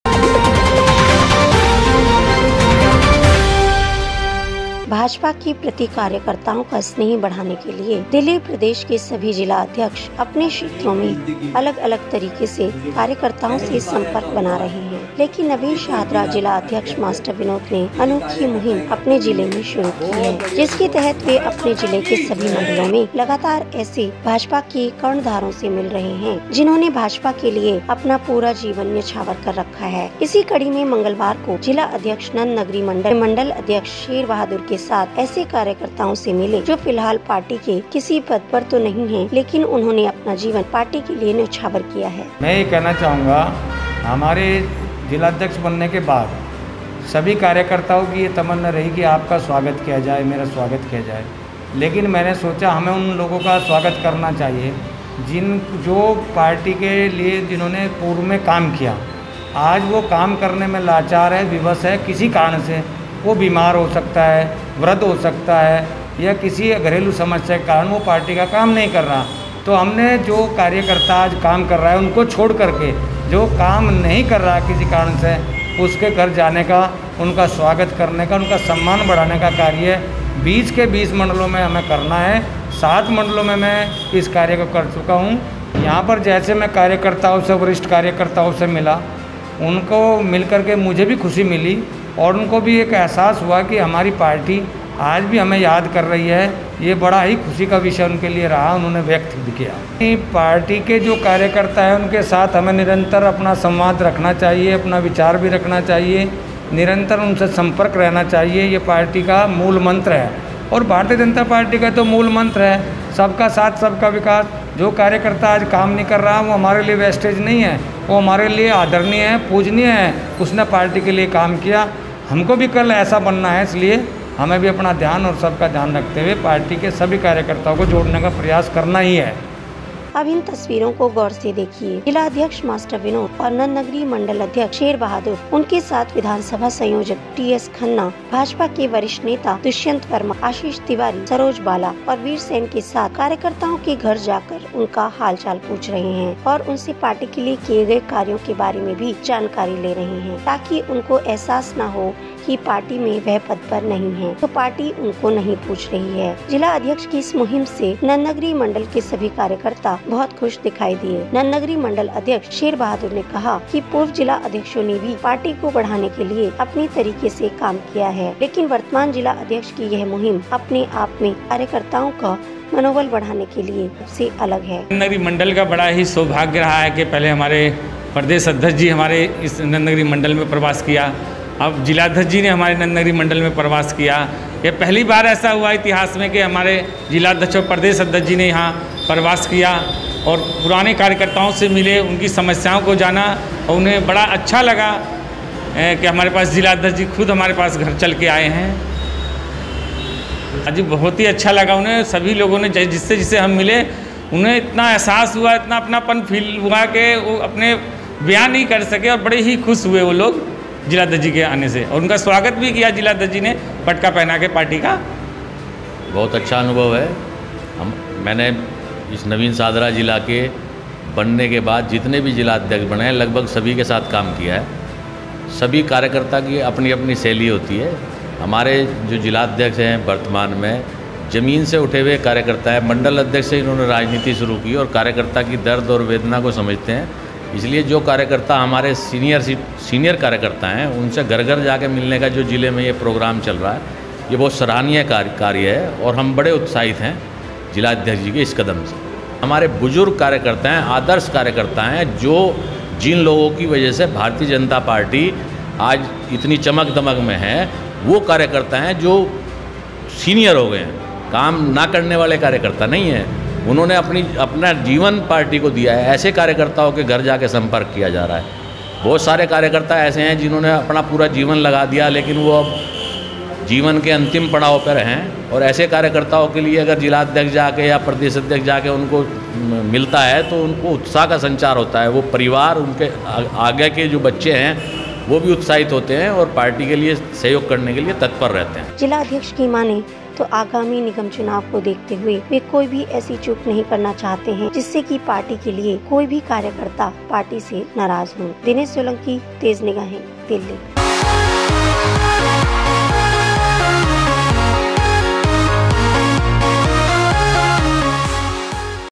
TejNews:- नंद नगरी मंडल के कार्यकर्ताओं से मंडल अध्यक्ष के साथ मिले जिला अध्यक्ष मास्टर विनोद...
भाजपा के प्रति कार्यकर्ताओं का स्नेह बढ़ाने के लिए दिल्ली प्रदेश के सभी जिला अध्यक्ष अपने क्षेत्रों में अलग-अलग तरीके से कार्यकर्ताओं से संपर्क बना रहे हैं .लेकिन नवीन शाहदरा जिला अध्यक्ष मास्टर विनोद ने अनोखी मुहिम अपने जिले में शुरू की है. जिसके तहत वे अपने जिले के सभी मंडलों में लगातार ऐसे भाजपा के कर्णधारों से मिल रहे हैं... जिन्होंने भाजपा के लिए अपना पूरा जीवन न्योछावर कर रखा है...